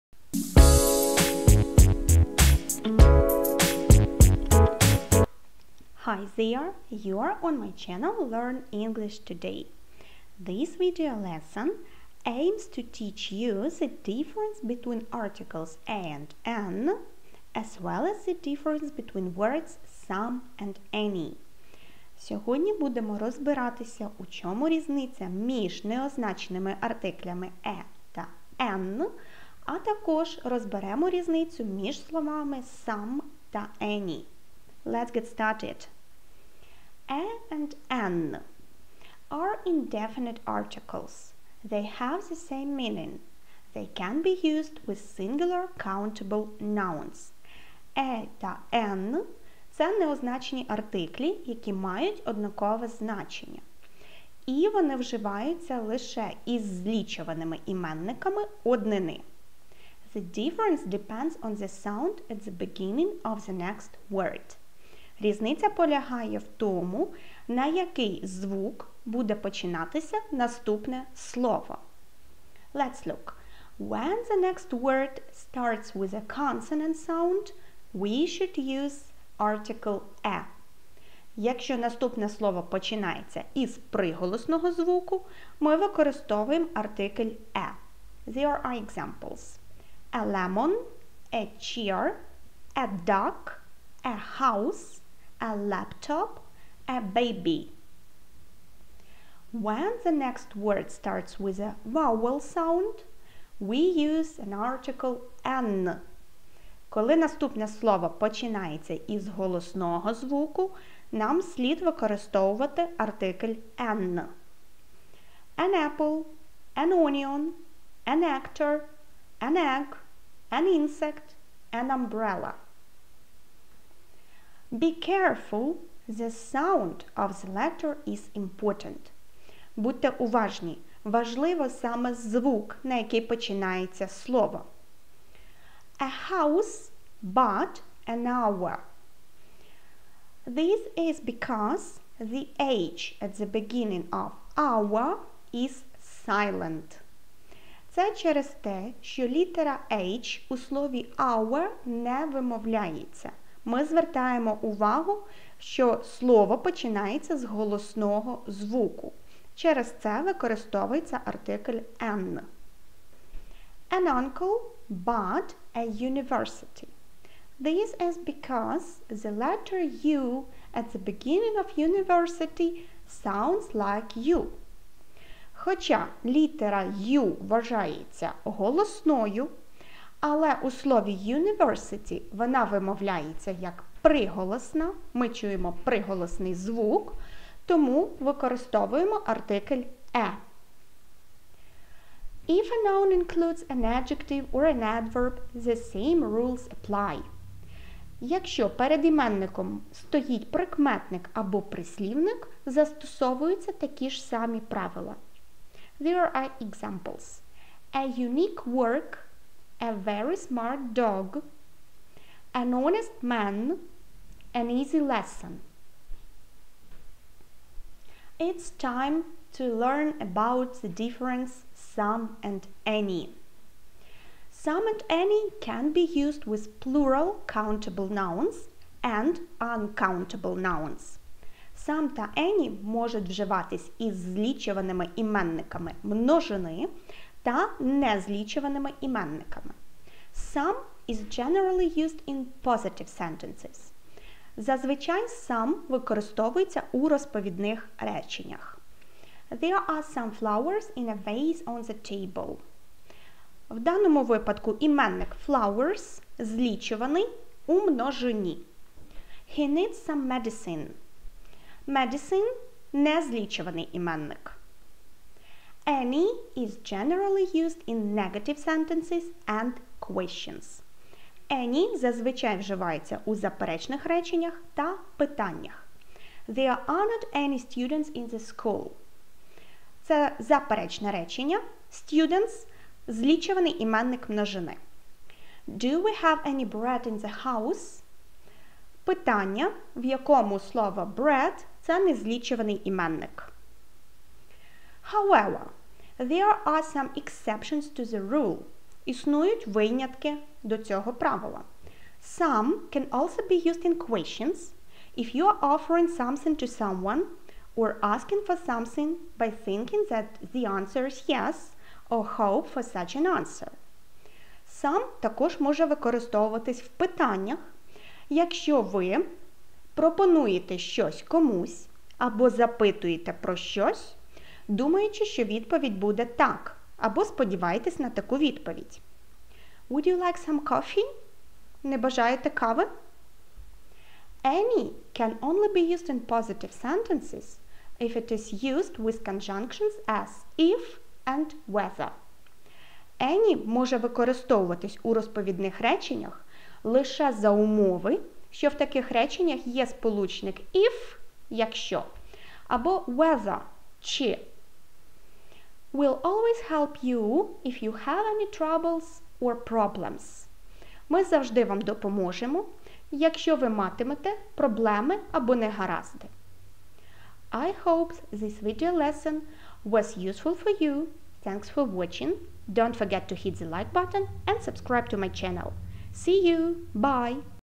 A/an; some/any - grammar rules and example sentences. Правила,приклади речень та зрозуміле пояснення
When to use indefinite articles a/an. The difference between some and any.

Table of contents:
00:00 Introduction
00:36 Articles a/an
03:37 Difference between some and any
04:54 Exceptions to the rule